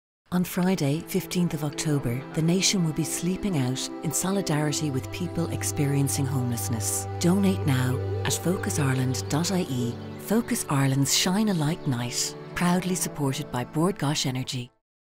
Bord Gáis Energy - Shine a Light 2021
Bord Gáis Energy are proud sponsors of Focus Ireland ShineALightNight again this year. We’ll be sleeping out on Friday 15 October to raise vital funds to help end homelessness in Ireland.